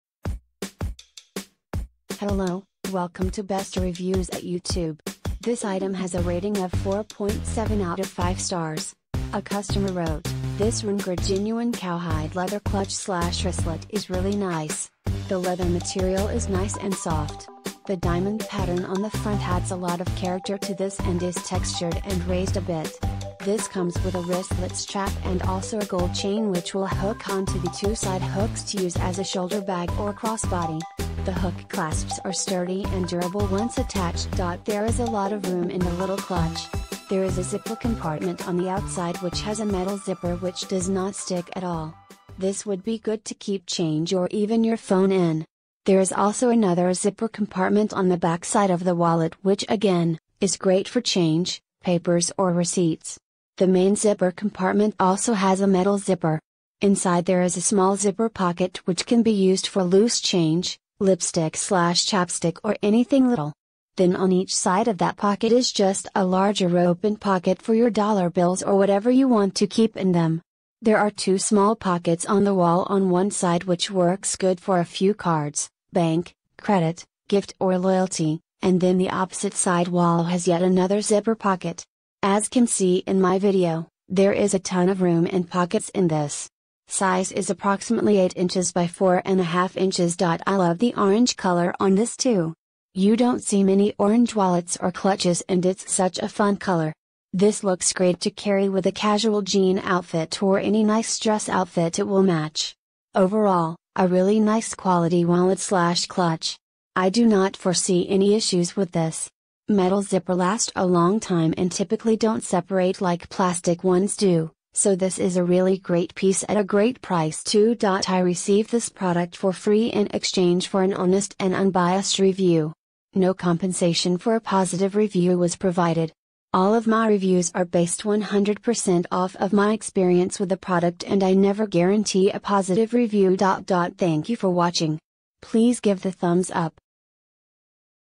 Rnker Multi-purpose Genuine leather women wallets /cross body bag /clutch wallet/wristlet clutch /
Review about   Rnker Multi-purpose Genuine leather women wallets /cross body bag /clutch wallet/wristlet clutch / handbags Wallet /shoulder bag /cellphone Case fit for iphone 6 , Galaxy S6, all cellphones (Yellow)